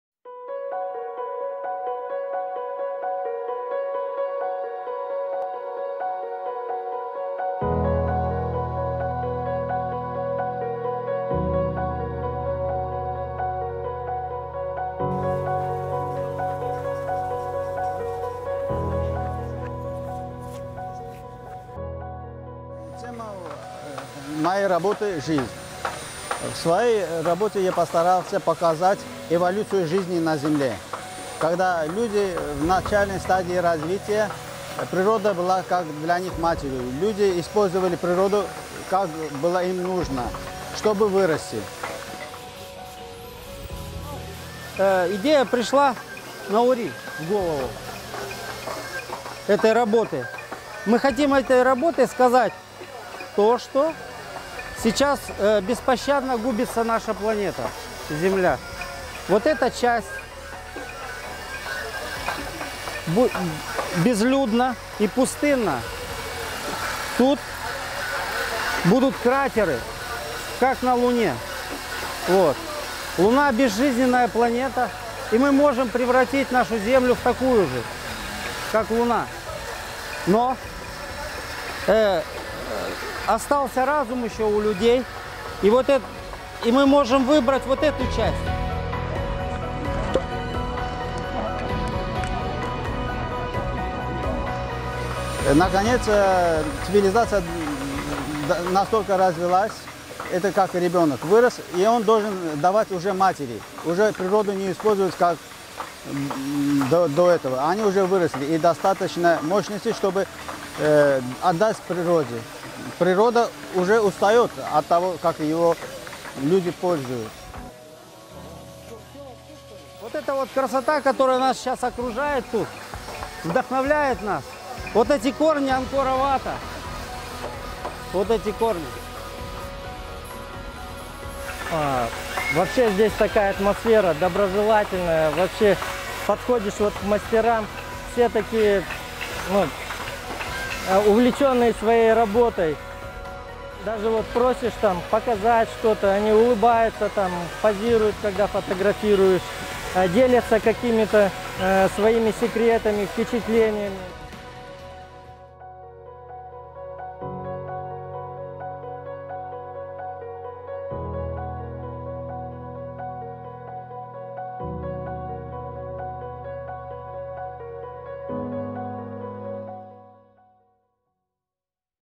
Life (2018WWD - Woodcarving)
2018 World Wood Day woodcarving video - A team from Armenia, Kazakhstan and Kyrgyzstan.
In the work, Nairi Safaryan, Vladimir Mossin and Pavel Kolodov express how the LIFE keeps on when people give back to nature than exploiting it.
In the beginning of the development of human, nature is like our mother. Human can use nature when they need it for developing. In the end, the civilization should give back something to mother nature in return.